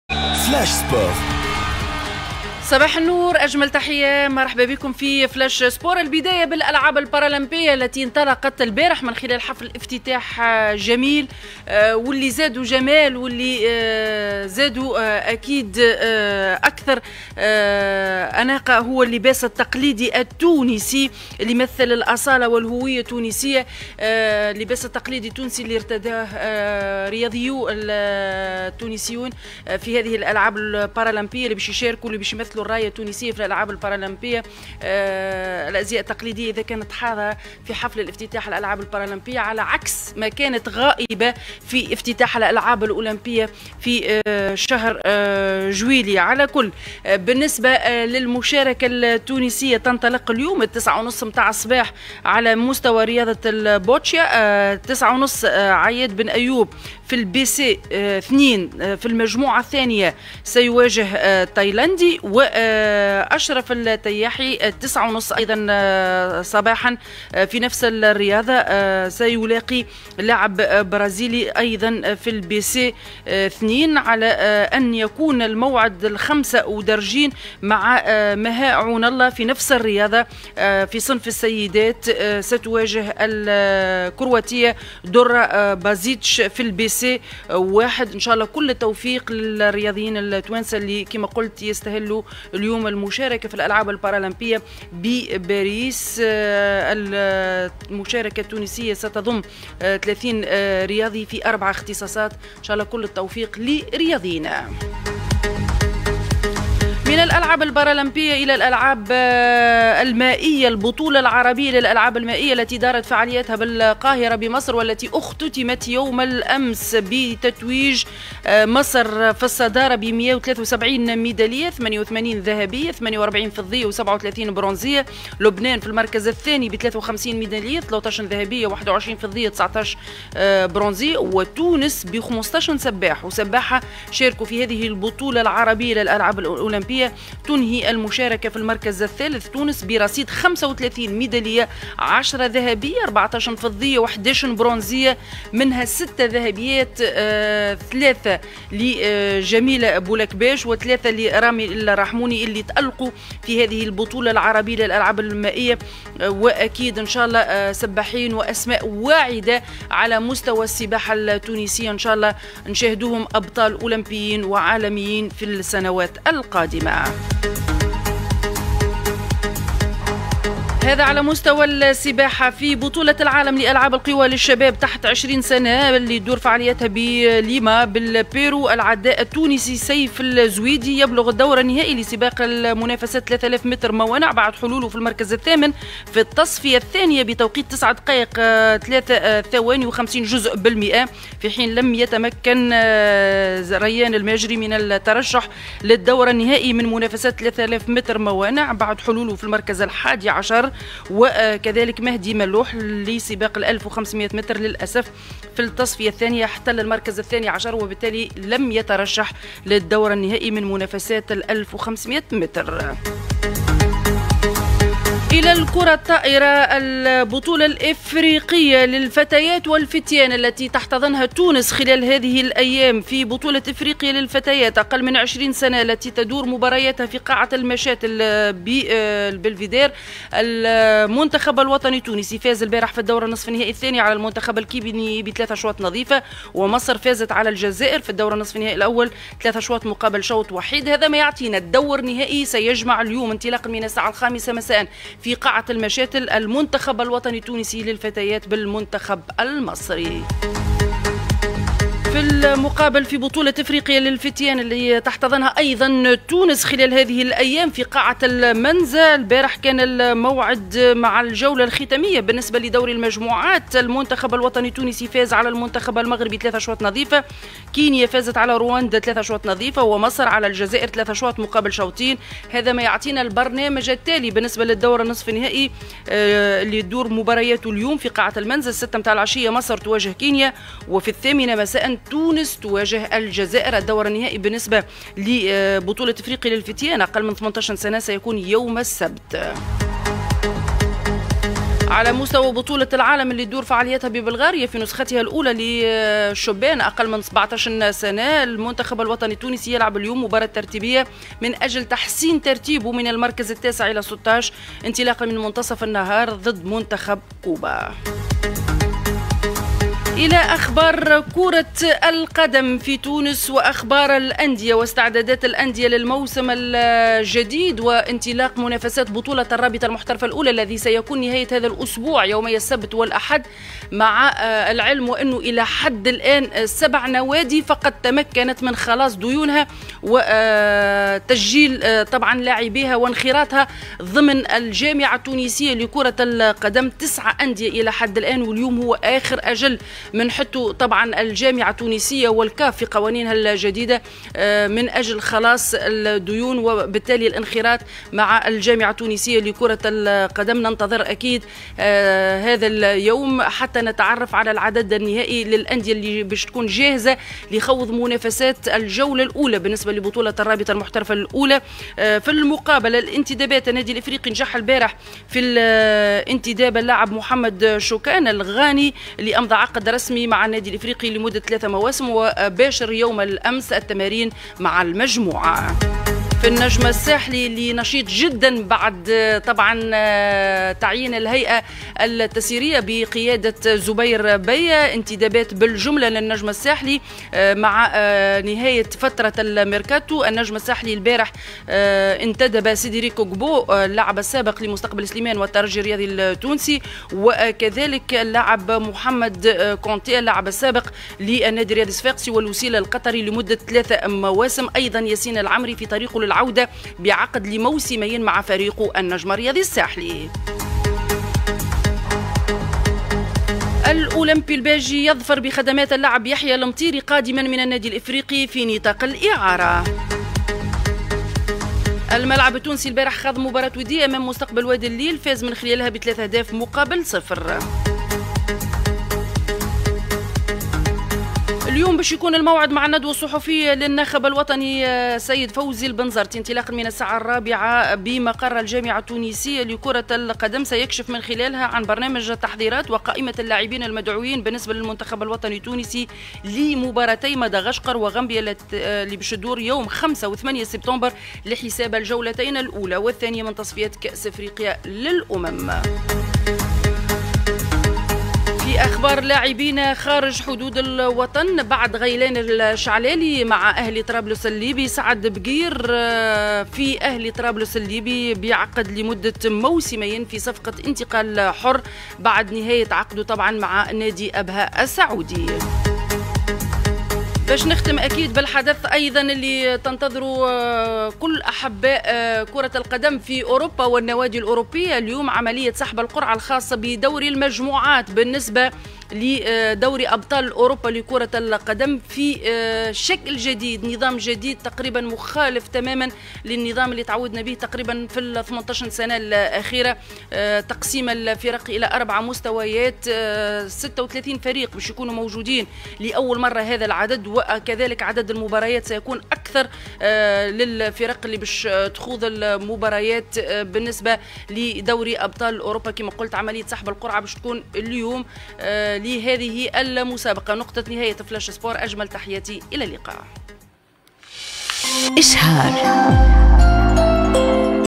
أخبار الرياضة | ليوم 29 ـ 08 ـ 2024
فلاش_سبور مع رجاء المختار
✔افتتاح دورة الألعاب البارالمبية باريس 2024
✔البطولة العربية للسباحة مصر 2024
✔النادي الافريقي: انتداب الغاني شوكان الى غاية جوان 2027